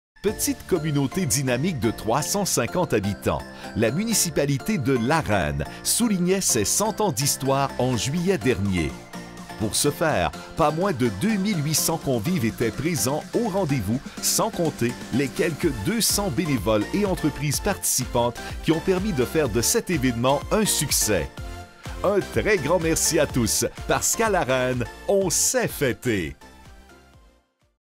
Comité du 100e de La Reine - Gala Excell'Or 2017
Finaliste dans les catégories "Événement" et "Coup cœur du jury" du Gala Excell'Or 2017 de la Chambre de commerce et d'industrie d'Abitibi-Ouest.

Petite communauté dynamique de 350 habitants, la Municipalité de La Reine soulignait ses 100 ans d’histoire en juillet dernier. Pour ce faire, pas moins de 2 800 convives étaient présentes au rendez-vous sans compter les quelques 200 bénévoles et entreprises participantes qui ont permis de faire de cet événement un succès.

Un très grand merci à tous parce qu'à La Reine, on sait fêter!